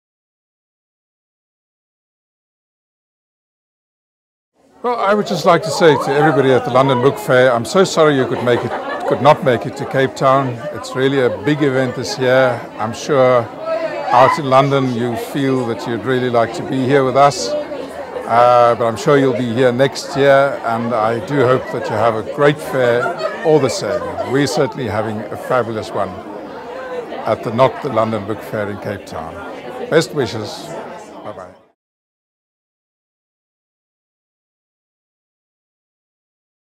Frederik de Jager, Umuzi
The eruption of a volcano in Iceland in April 2010 spewed a volcanic ash cloud over much of Europe, grounding airlines and stranding passengers.

Among the many thousands unable to travel were South African authors and publishers due to have flown to London for the 2010 London Book Fair which had chosen South African literature as its focus.

On the first day of the book fair, publishers and authors gathered at the Book Lounge in Cape Town to celebrate the spirit of the book fair and offer messages of support to those in London.

Here is a message of support from Frederik de Jager of Umuzi publishers